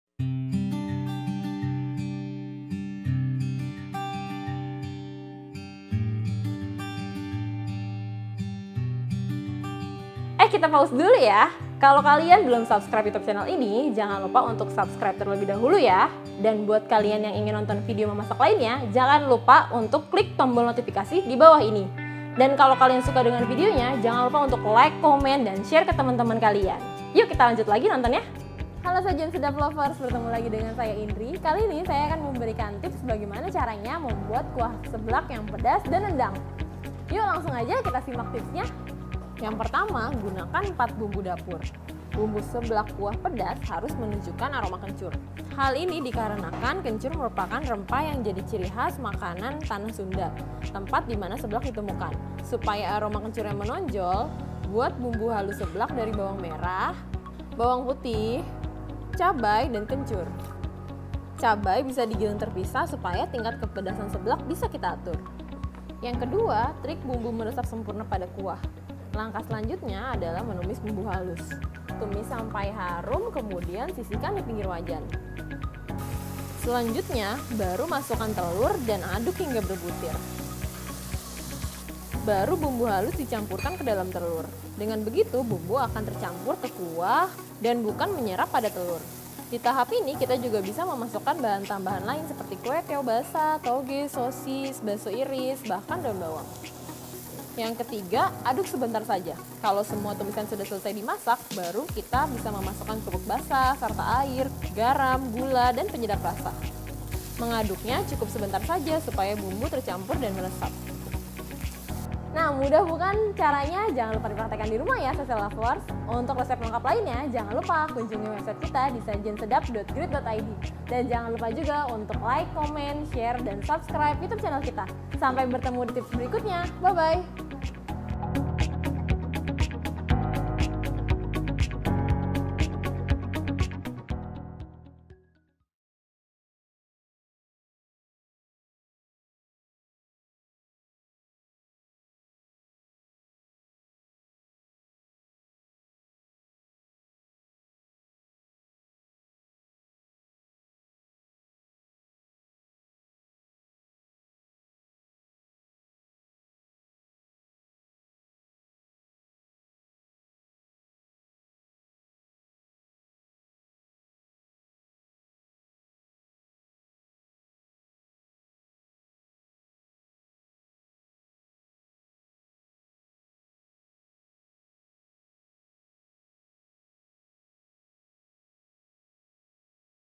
Cara Membuat Kuah Seblak yang Enak, Gurih dan Harum Banget
Bikin seblak sendiri di rumah? Belajar dulu cara membuat kuah seblak yang enak, gurih dan harum banget.

00:00- intro
00:31- opening
00:40- bumbu kuah seblak
01:10- cara membuat kuah seblak
01:54- closing

Produser: Intan Yusan Septiani
Penulis Naskah: Qorin & Indri
Peraga: Qorin, Resi & Indri
Fotografer: Bambang Tetuko, Andrian & Osyad
Videografer: Bambang Tetuko, Andrian & Osyad
Video Editor: Bambang Tetuko, Andrian & Osyad